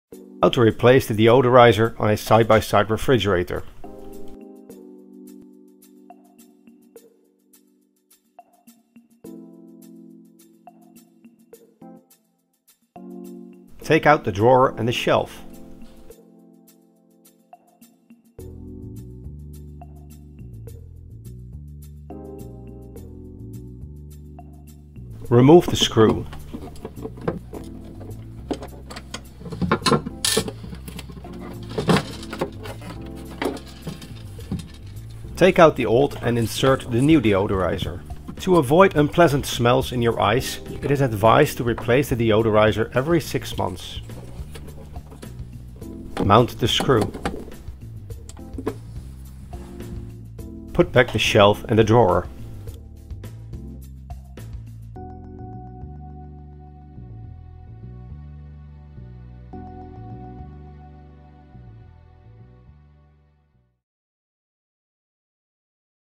[LG Refrigerator] - Replace the Air filter (deodorizer) on a Side-by-Side model
This video explains how you can replace the Air filter (deodorizer) on a Side-by-Side model.
Note that the filter location and (dis)assembly may differ per model.

Reuse/Replace the filter:
the Air filter (deodorizer) can be re-used. You can dry the filter under direct sunlight or dry it using a hair dryer.
In case you notice bad smells or taste in your ice cubes, replace the filter.
In some cases it is not possible that smells remain in the filter, even after drying.
Then replace the filter.
Part number: MCS62101805 (MCS62101805, 70 x 100mm. Filter may differ per model so check your air filter size first)